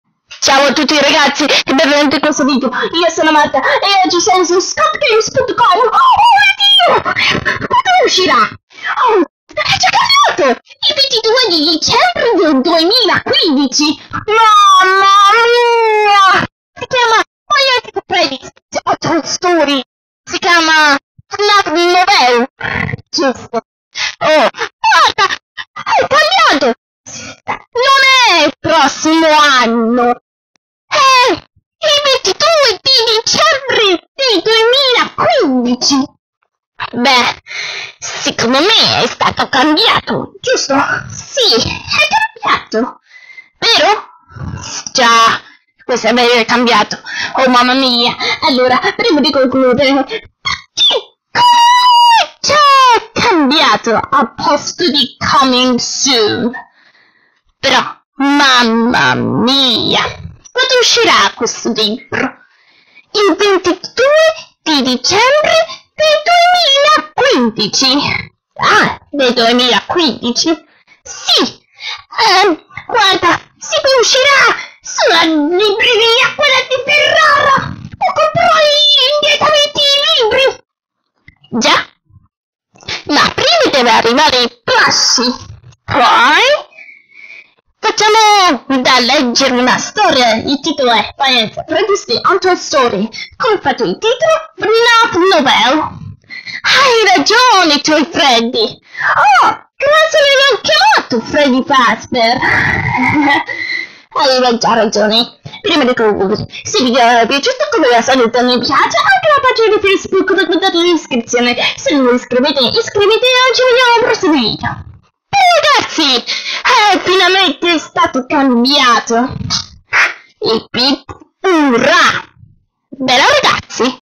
La data a posto di "coming soon"
Ieri la data è al posto di "coming soon" su scottgames.com